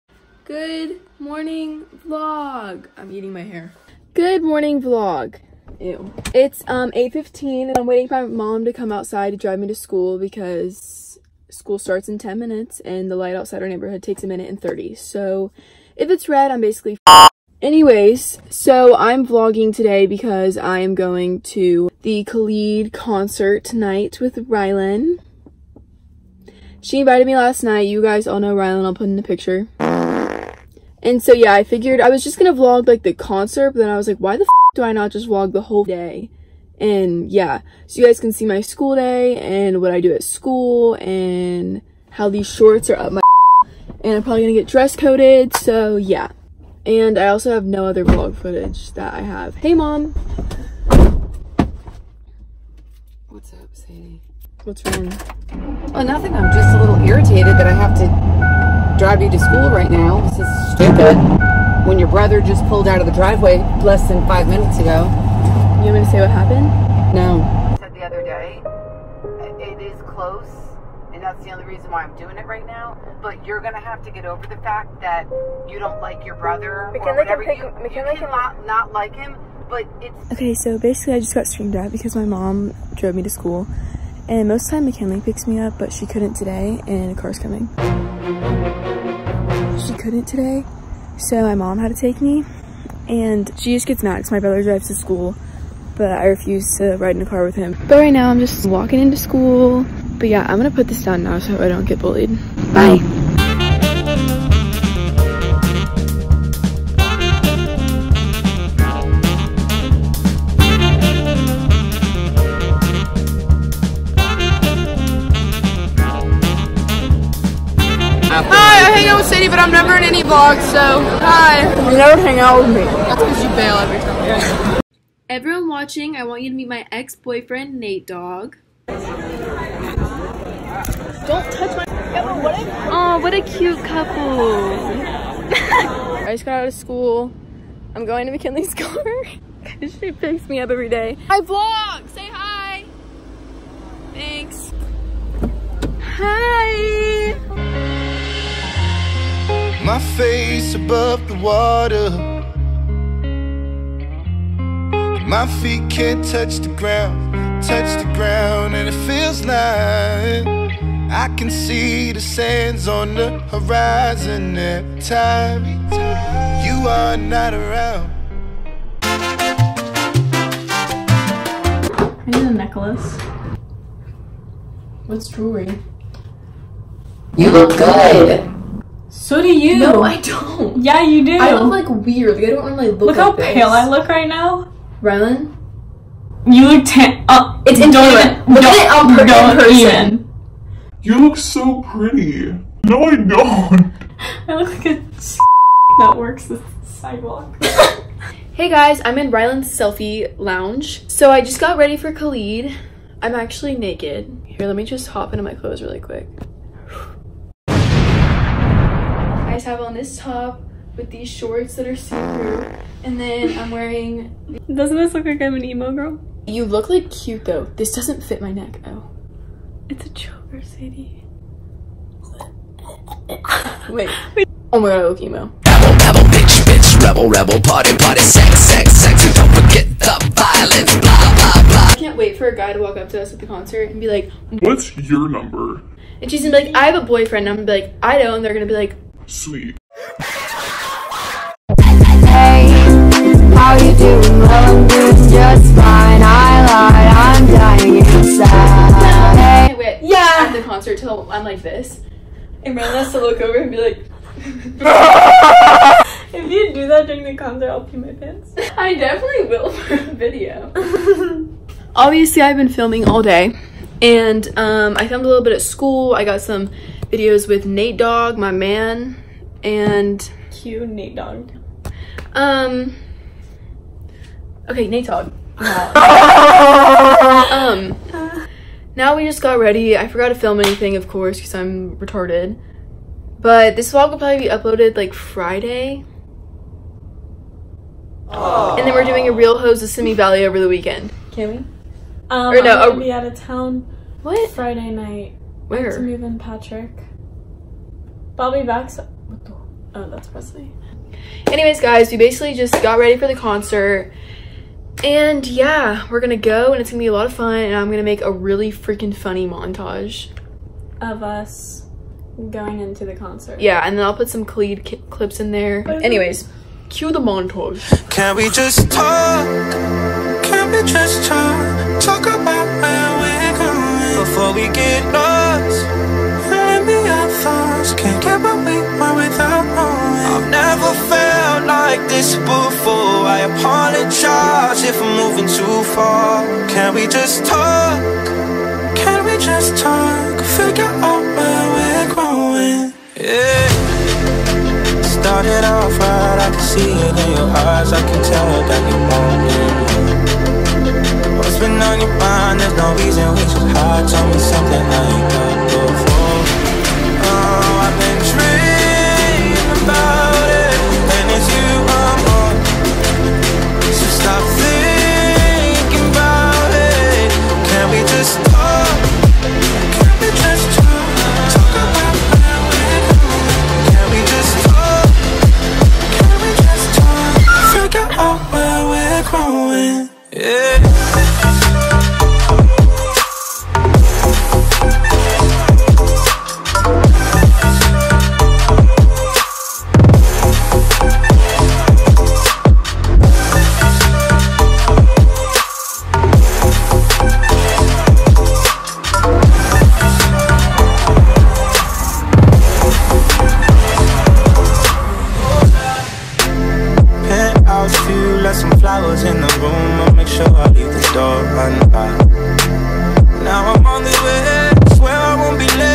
MEETING KHALID?!? KHALID CONCERT VLOG!!!
Thank you rilyn for bringing me! I'm so happy you and Natedawg are beginning to become more than friends love you :)